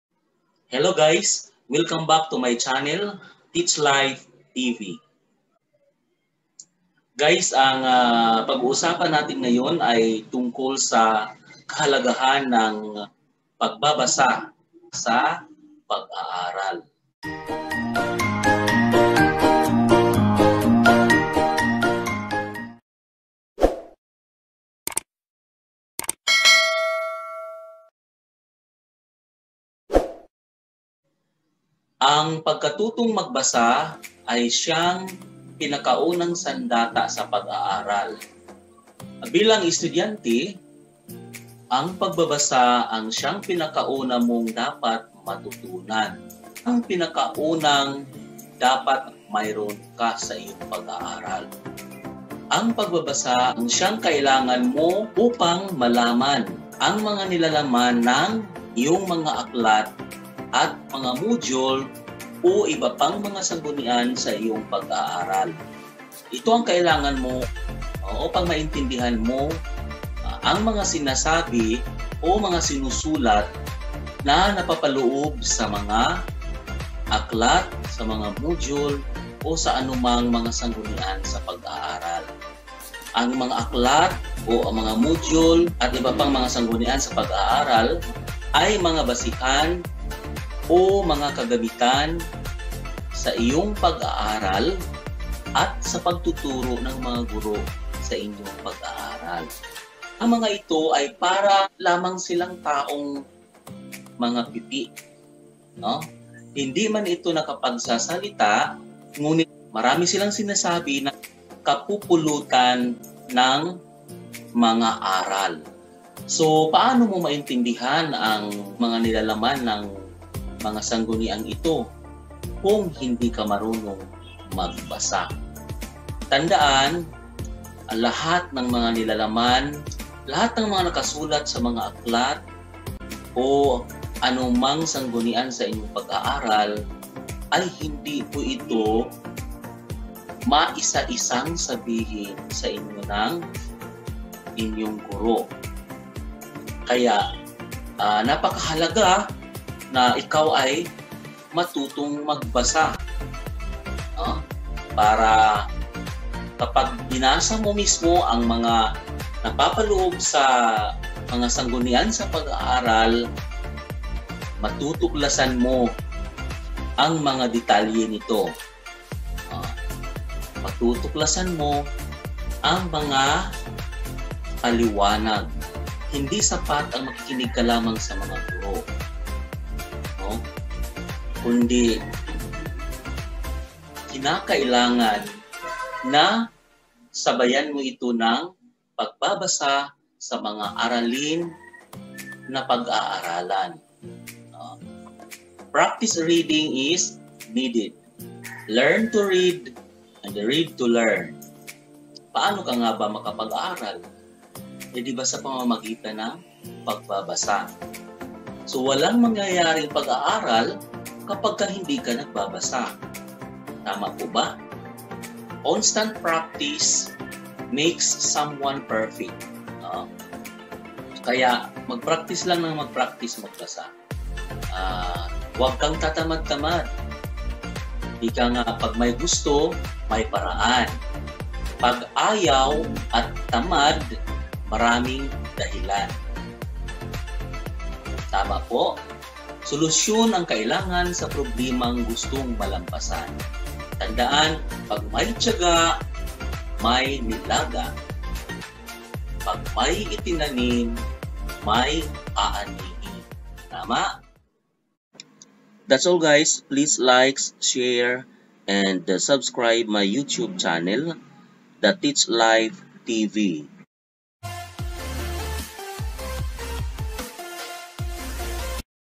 ANG KAHALAGAHAN NG PAGBABASA SA PAG-AARAL (A seatdown talk)
Ang videong ito ay nagpapaliwanag kung gaano ka importante ang pagbabasa sa pag-aaral.